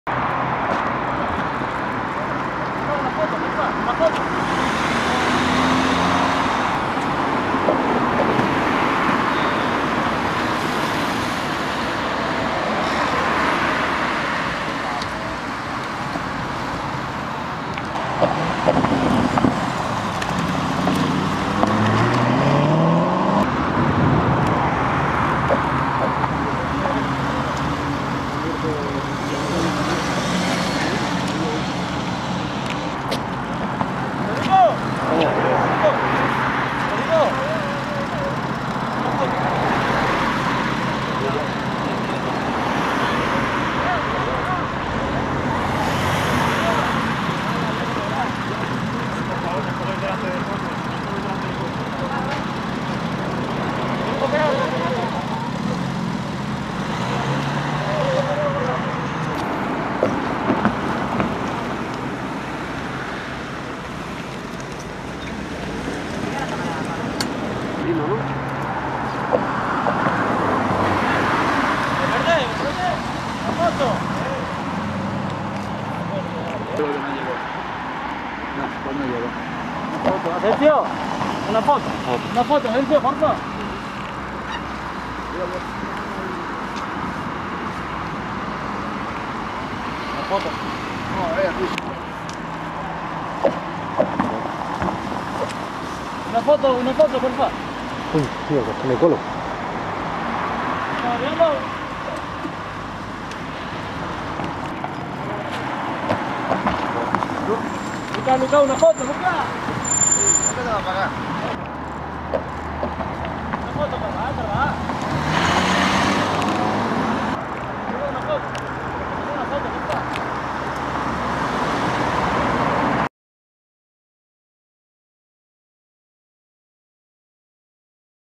Los jugadores del Real Madrid abandonan Valdebebas tras el entreno previo al Clásico
Barça y Real Madrid se enfrentarán en el partido de la jornada 30 de LaLiga. Los jugadores de Zidane han entrenado en Valdebebas y luego han abandonado el campo de entrenamiento en sus coches particulares para preparar el encuentro decisivo contra su eterno rival.